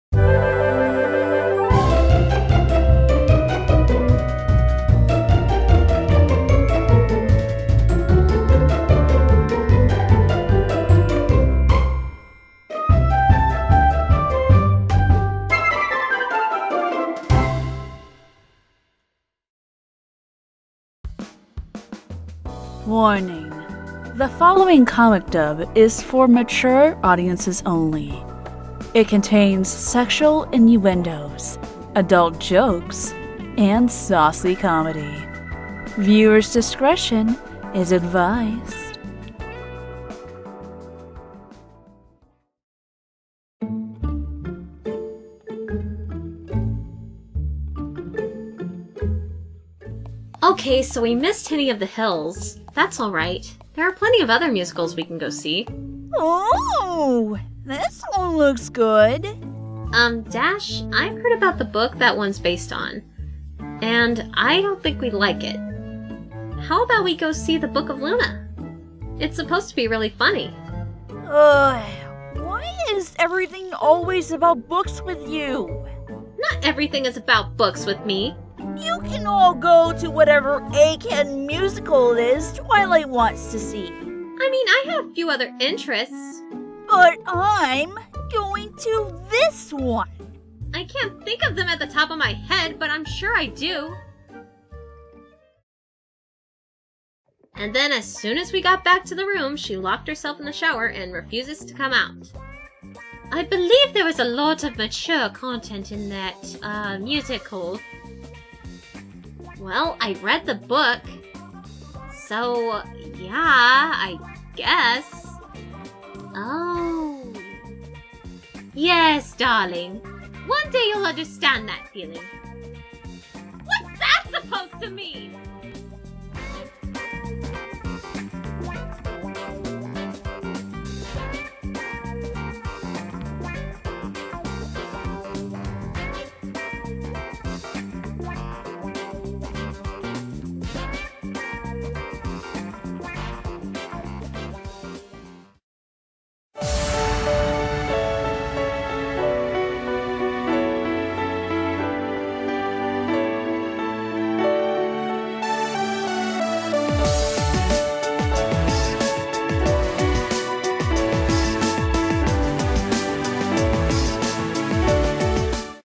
Feeling Dirty MLP Comic Dub (Saucy Comedy)
Warning:

The following comic dub is for mature audiences only! It contains sexual innuendos, adult jokes and saucy comedy. Viewer's discretion is advised!

This is my first saucy comedy comic dub of 2022! I hope you all enjoy it!

Agent0Fluffy as Rainbow Dash